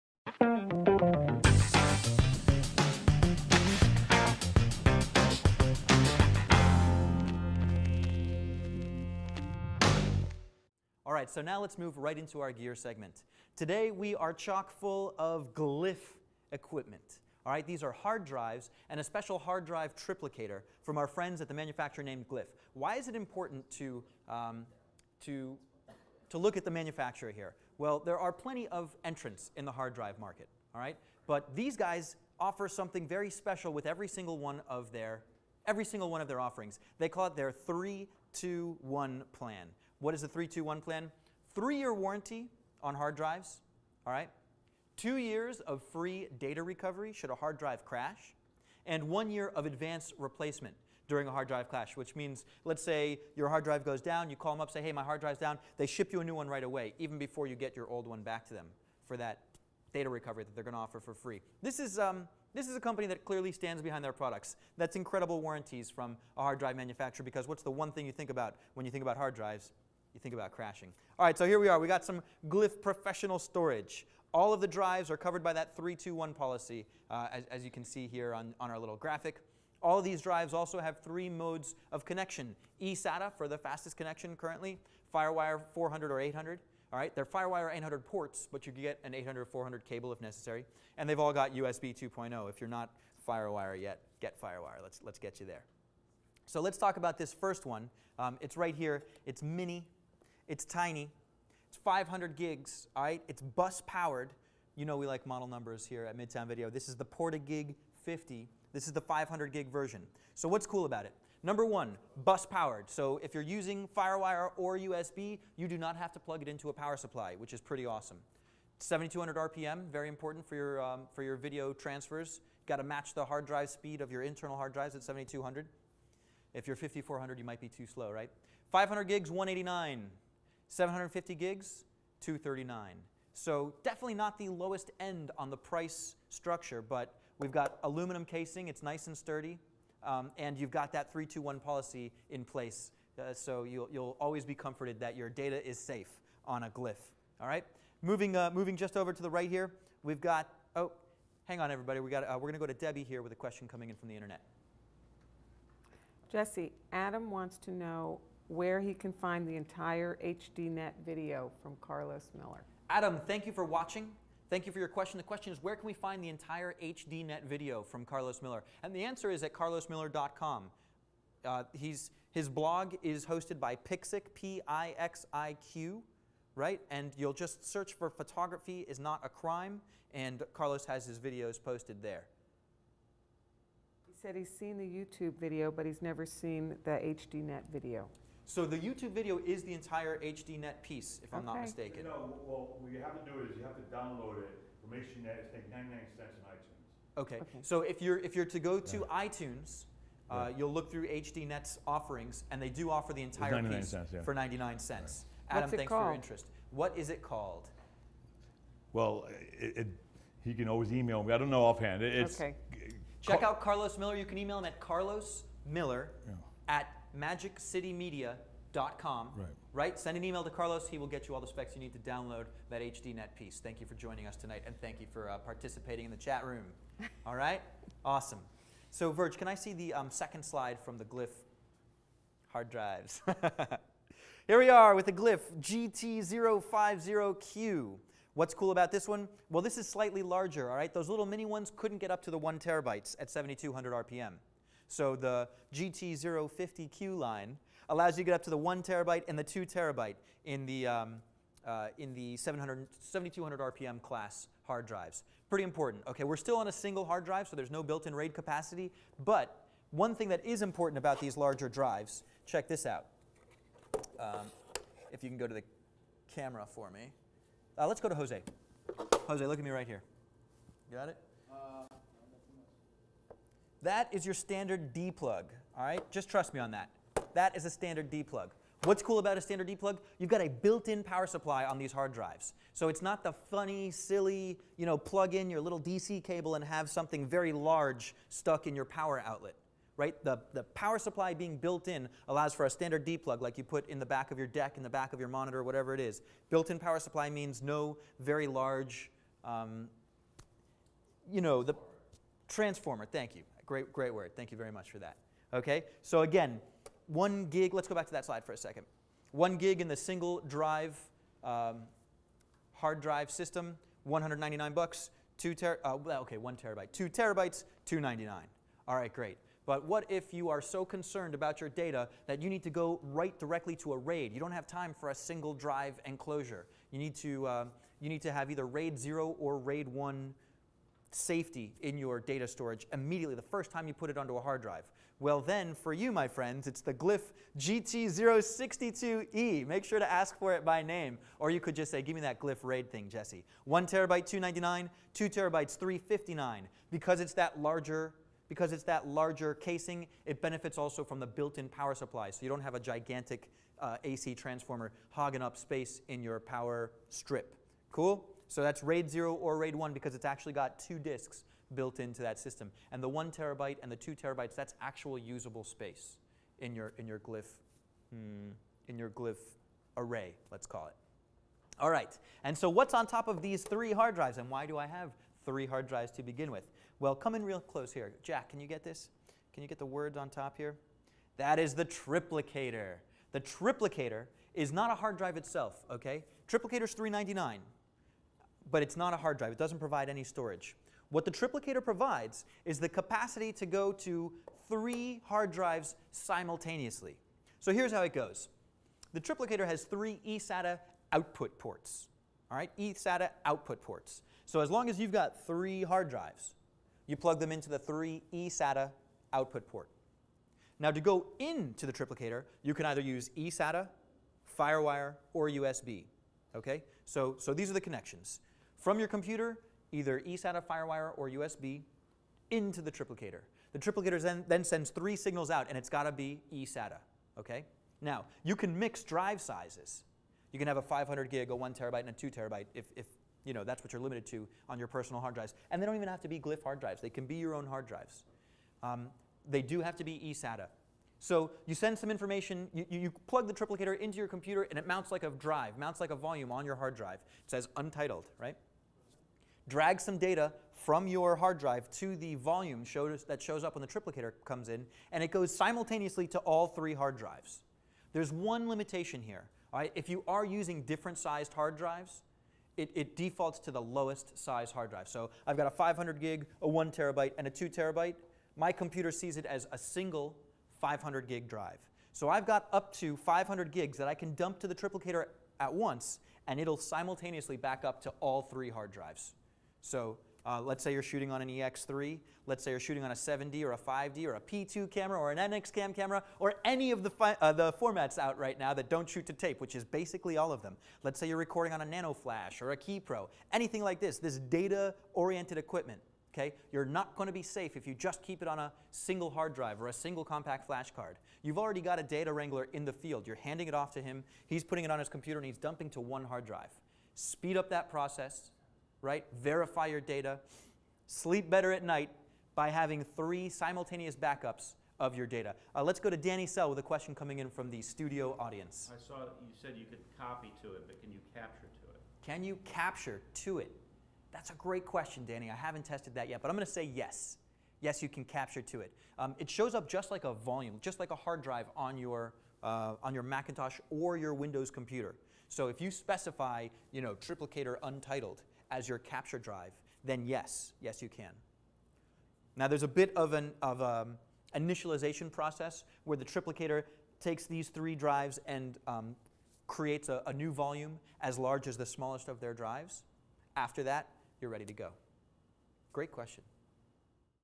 Glyph Hard Drives & Triplicator - ep 26
jtown.tv and midtown video present: "the .video show, with jesse miller".  this segment features three totally awesome hard drives from Glyph that set themselves apart from other hard drives on the market. we also feature the Glyph Triplicator, a time-saving device that will make your job easier by allowing you to make up to three simultaneous copies of your critical data onto three seperate hard drives.